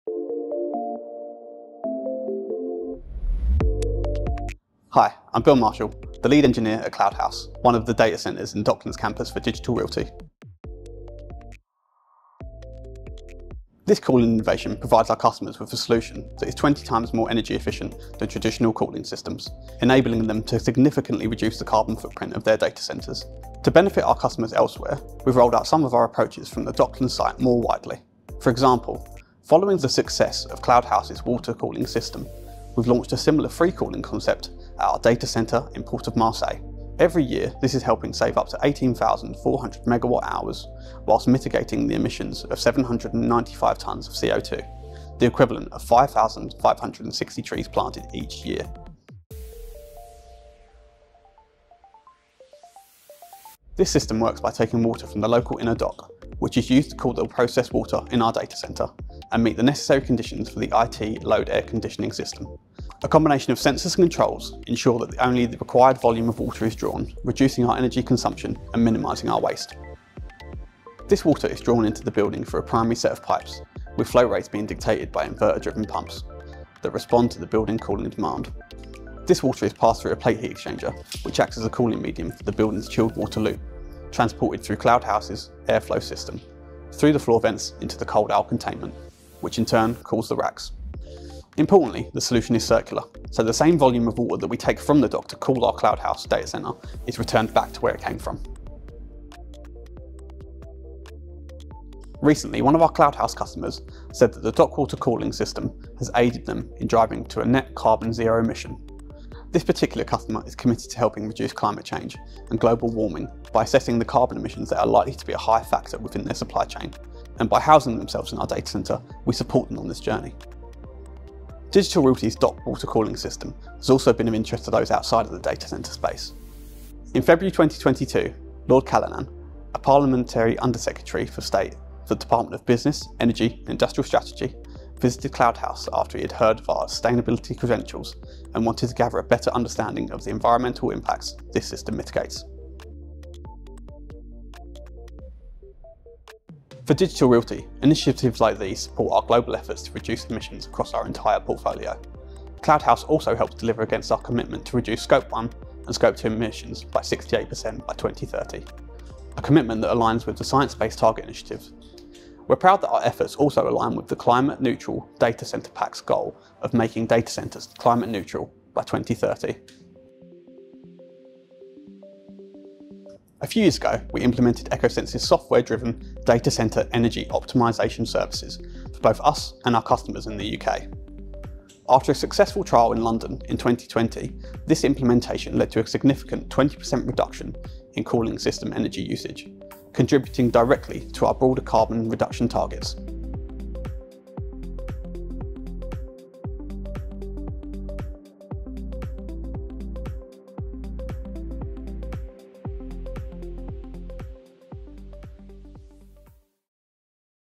Digital Realty’s Cloud House Data Centre in the London Docklands, UK
Learn about Digital Realty’s river dock cooling system at the Cloud House data centre in
London. It provides customers with a solution that is 20 times more energy efficient than traditional cooling systems, enabling them to significantly reduce the carbon footprint of their data centres.